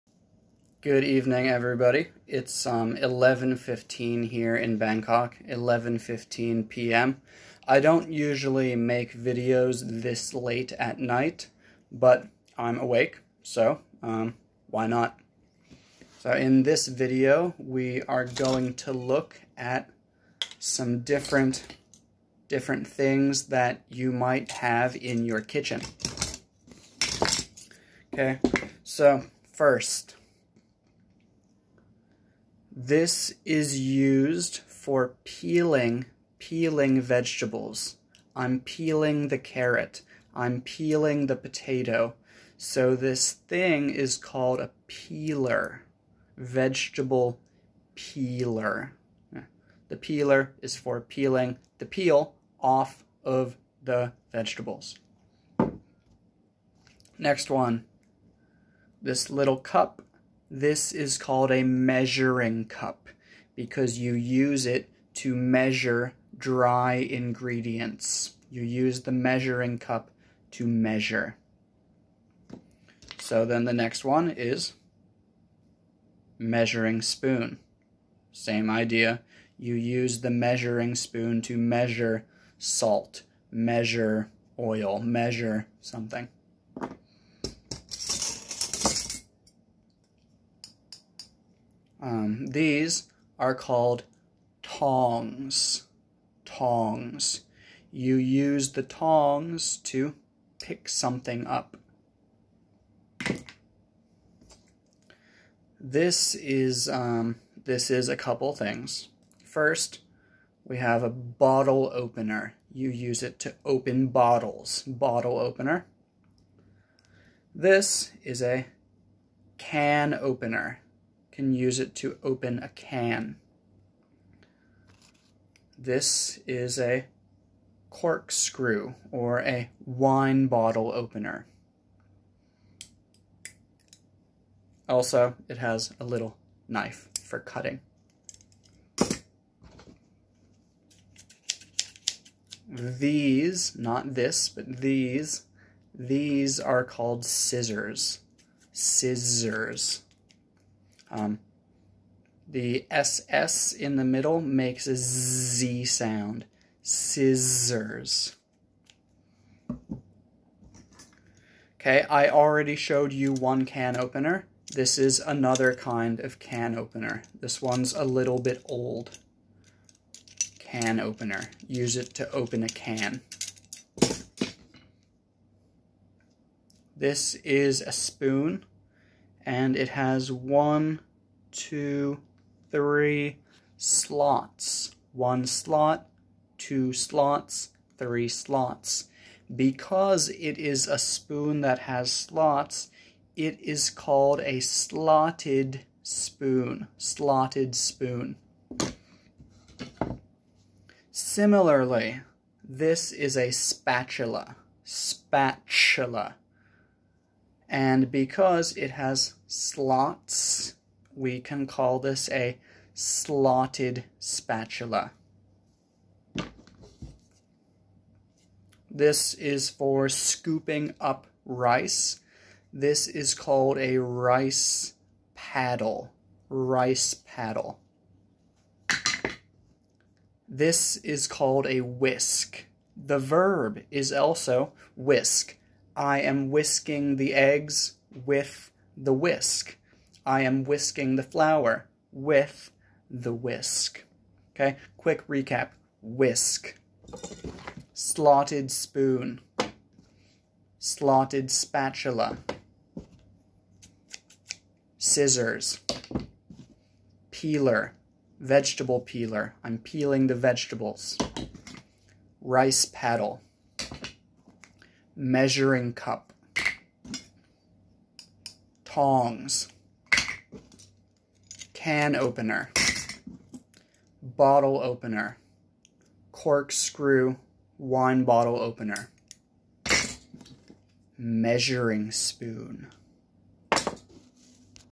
Do Know the English Names for These KITCHEN UTENSILS?!
Learn the English names for kitchen utensils (tools) and the things that we do with them.

-(Vegetable) peeler
-Measuring cup
-Measuring spoon
-Scissors
-Bottle opener
-Corkscrew / wine bottle opener
-Can opener
-Tongs
-(Slotted) spatula
-(Slotted) spoon
-Rice paddle
-Whisk